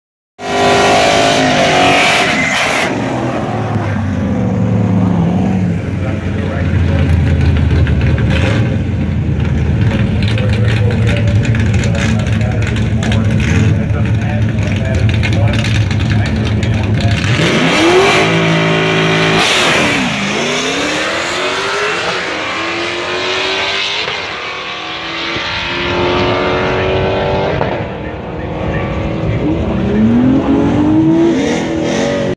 LT1 94 Camaro (11.54) vs C7 Corvette
11.54@119.87 pass in my 94 Camaro.

Major mods: ported stock heads, custom cam , suspension, 8.8 rear w/ 4.10's. 6000 rpm launch on 26"slicks

All motor, stock bottom end, 6 speed,  cutout open